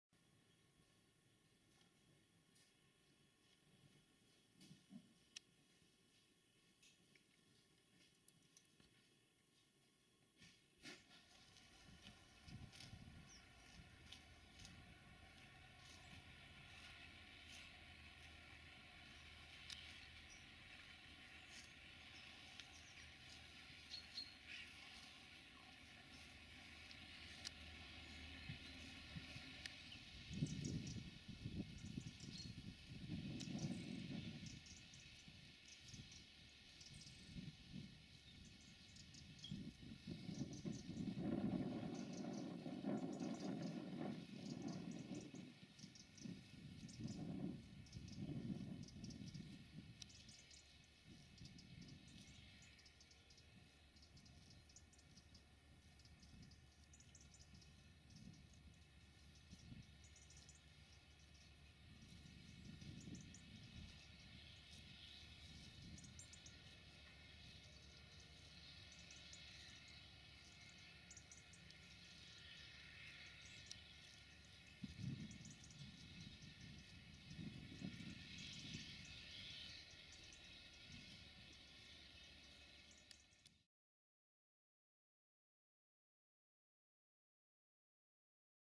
Weed-Killer Train Goes By, Spraying the Tracks.
Heard a "different" hoot for the level crossing and grabbed  my camera. Then this appeared. I knew it was in the area but not ben able to catch it till now.  Dirty beast!